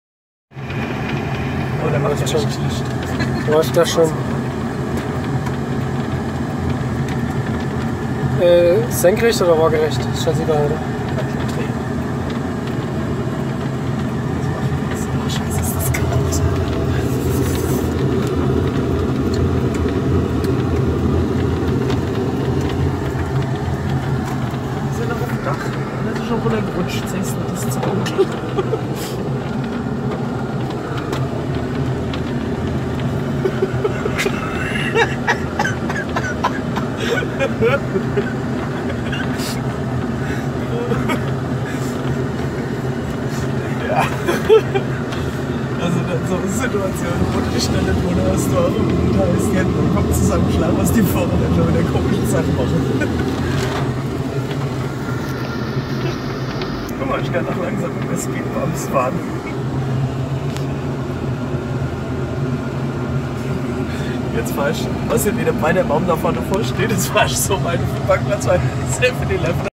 Phuket Dog Resort - Funny tree transportation
We got a new tree for our Resort and had to pick it up... heavy rain in that moment and the tree was in a way too big :D